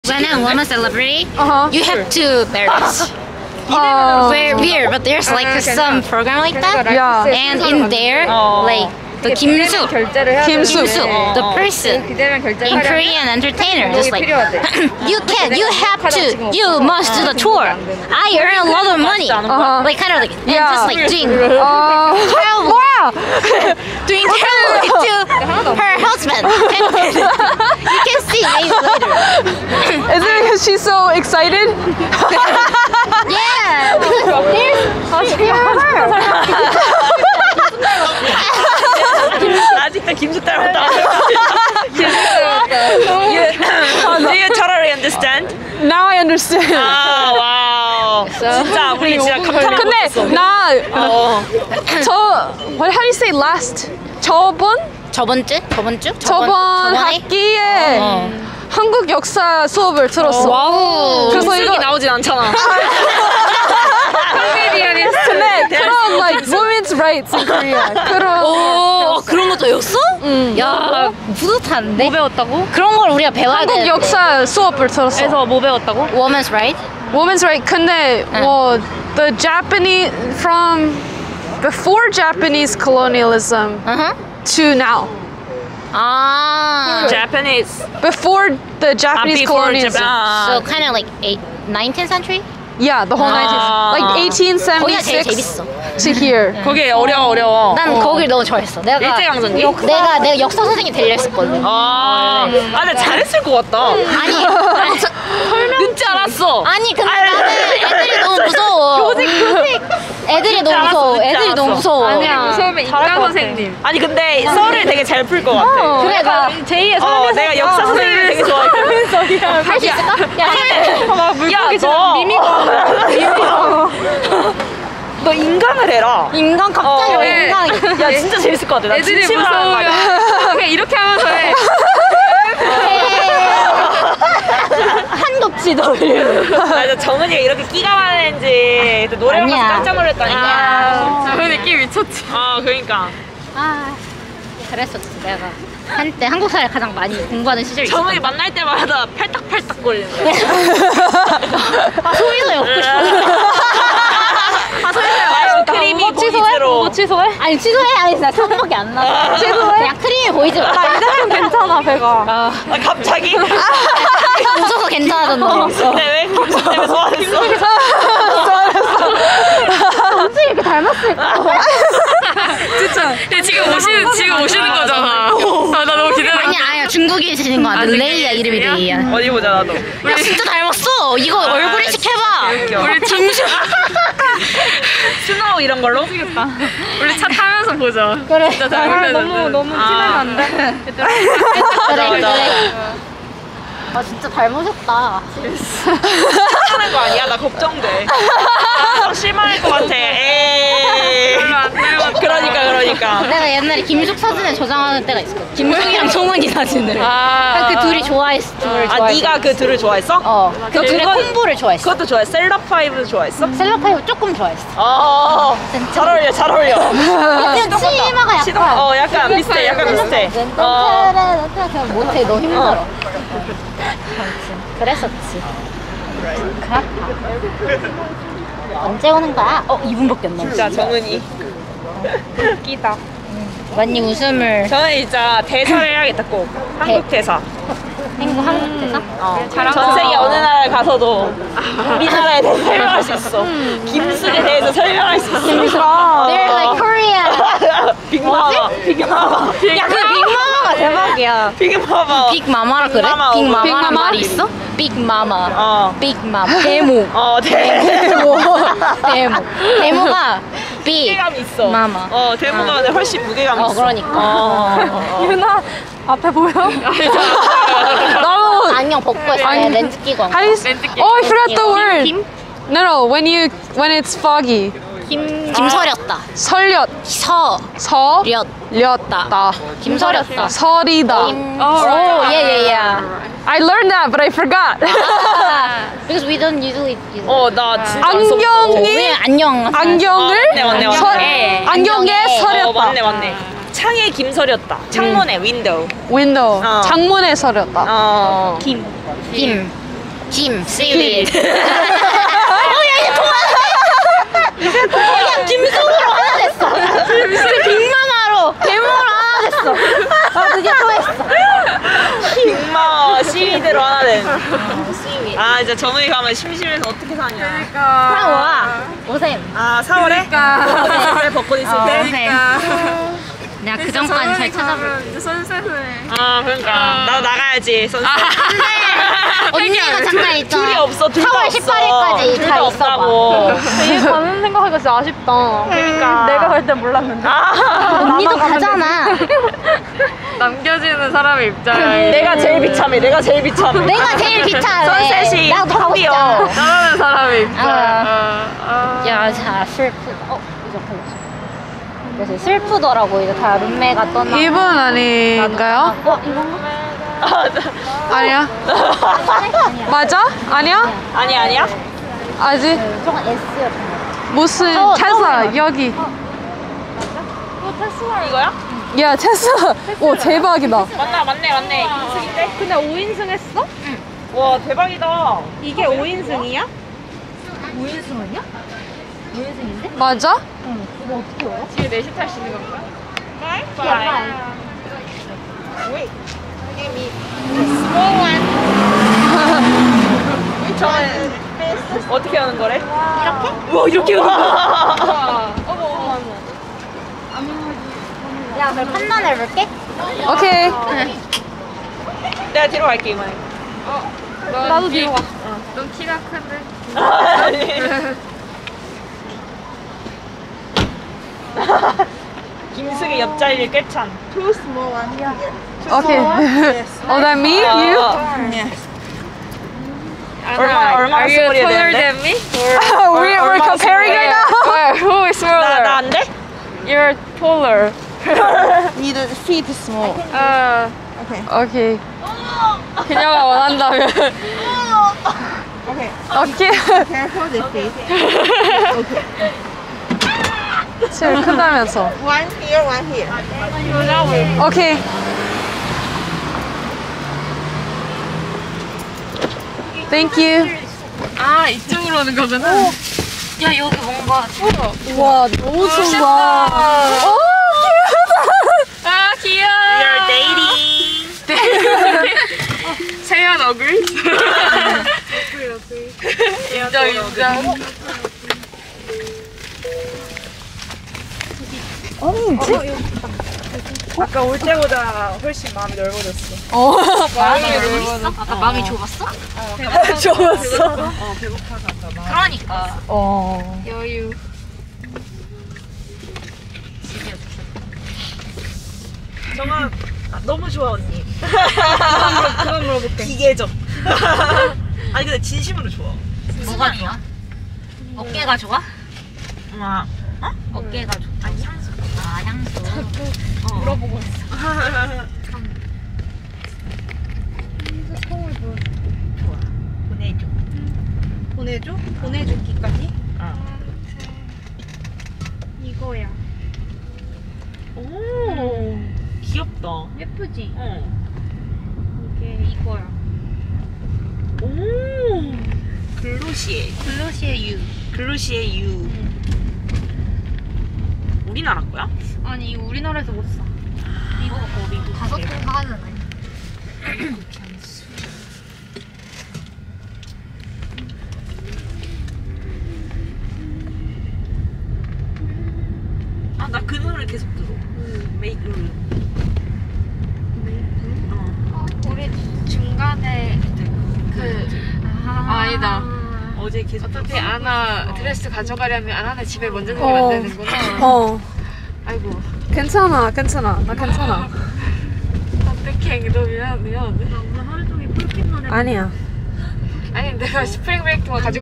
220313 waiting for the uber in sawtelle at 11pm - unedited
with 정은이, 유나 언니, 민혜 언니, 세현 언니 (편집 귀찮아서 안 함 too lazy to edit)